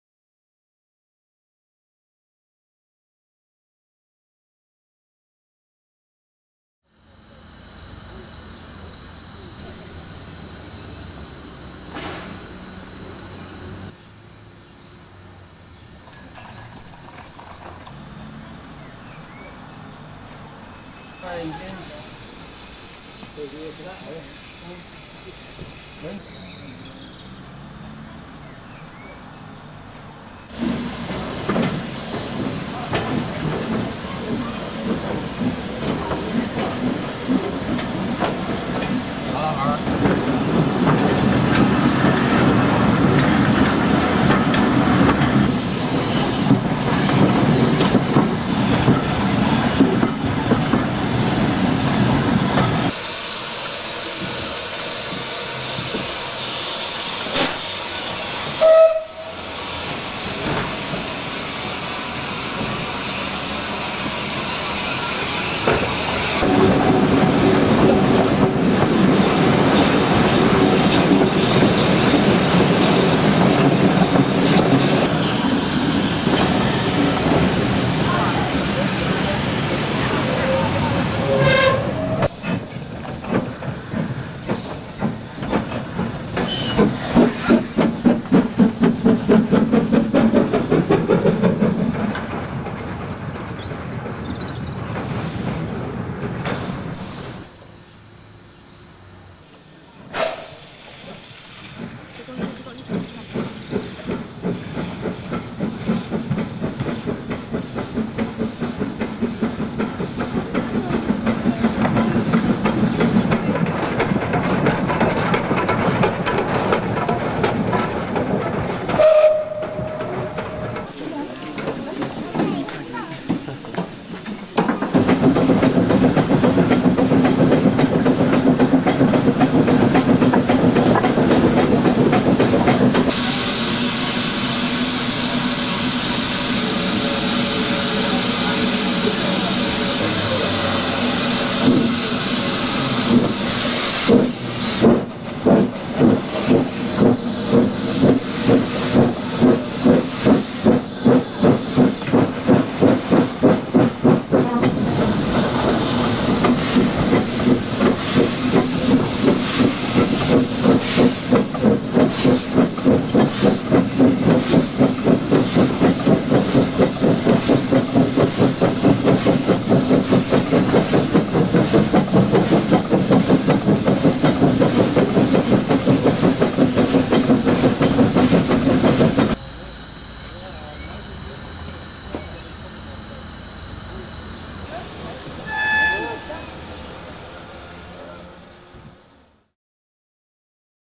Bodmin and Wenford Railway in April 1993 with saddle tank No. 19
We join Devonport Dockyard saddle tank No. 19 at Bodmin General station in April 1993 for a ride down the hill to Bodmin Parkway and back, stopping at Colesloggett Halt in both directions.